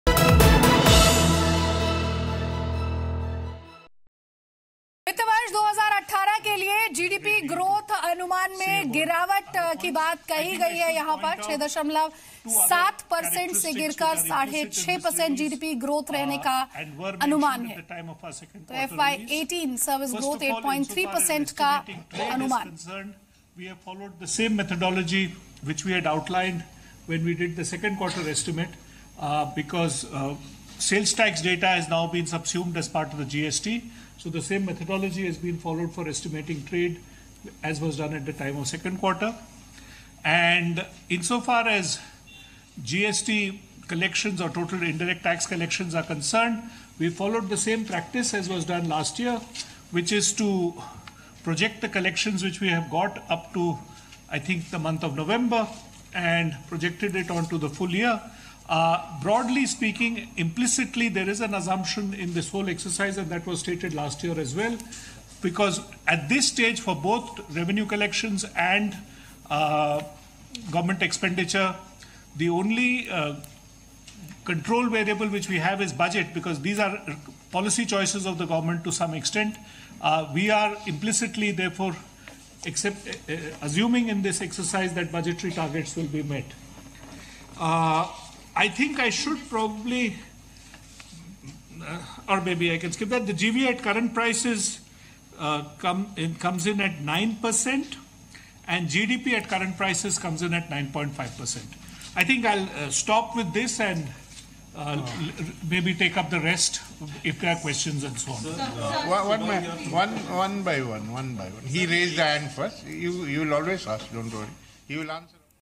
Manufacturing growth projected at 4.6 per cent for FY18
This segment of Zee Business brings to you latest news on growth projection for FY18. Watch this clip to know more.